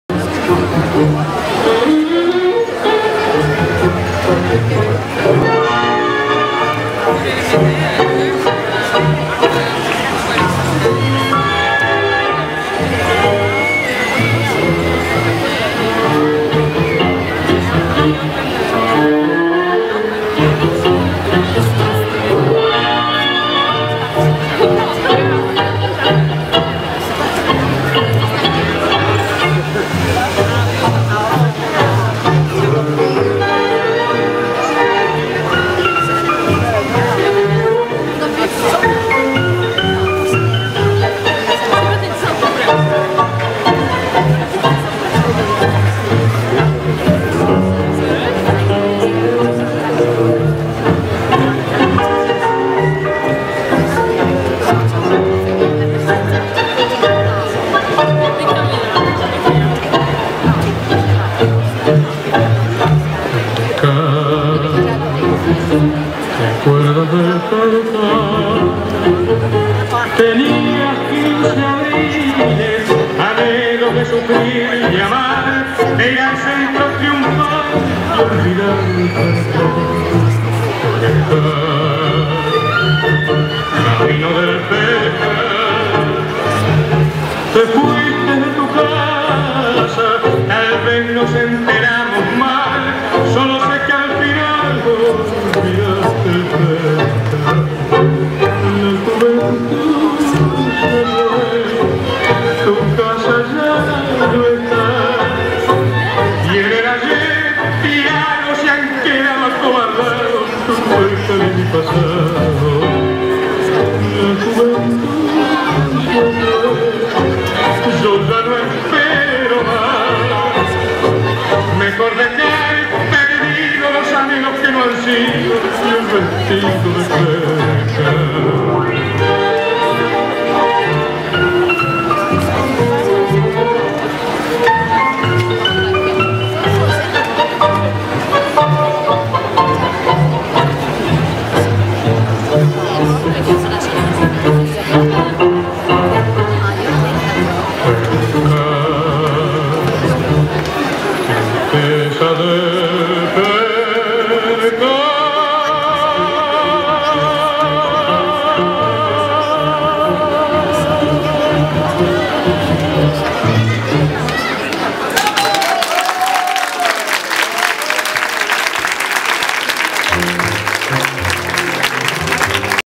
Tango with Giorgio Regnoli and Marianne Lingserius
A sunny evening Tango at Grönan